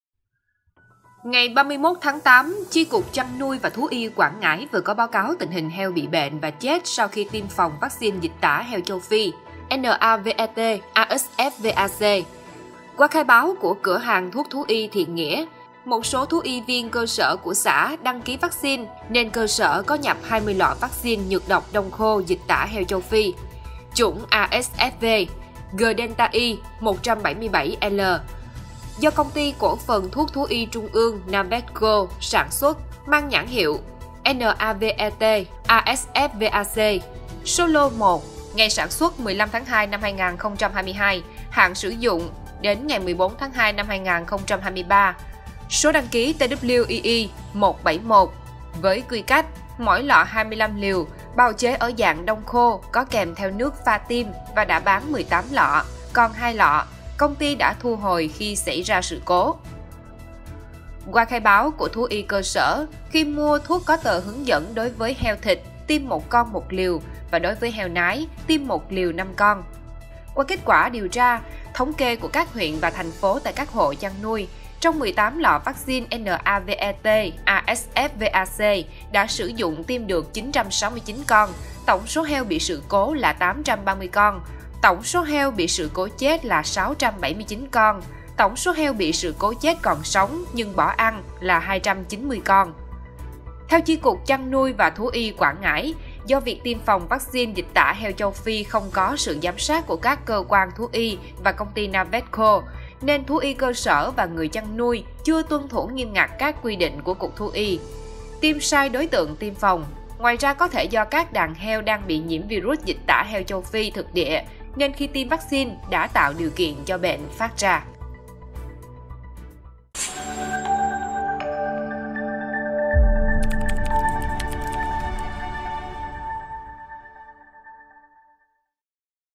Vì sao heo chết sau khi tiêm vaccine dịch tả heo châu Phi?
Ngày 31-8, Chi cục Chăn nuôi và Thú y Quảng Ngãi vừa có báo cáo tình hình heo bị bệnh và chết sau khi tiêm phòng vaccine Dịch tả heo châu Phi NAVET-ASFVAC.